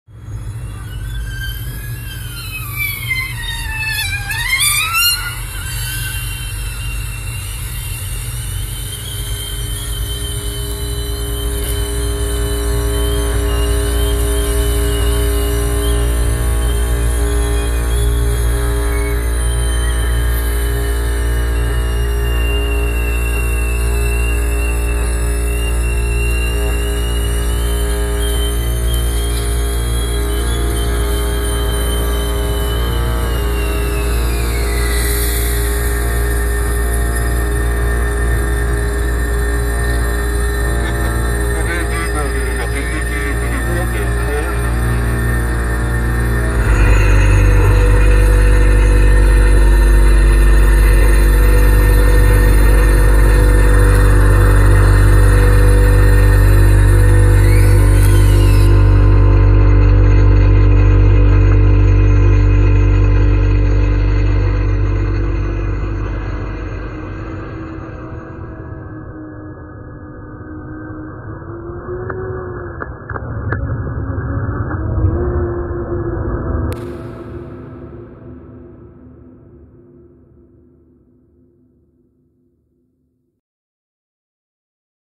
04.11.19 Coldharbourvangarageuk
The sound of a van transmission affecting the stereo + UK garage station that is off air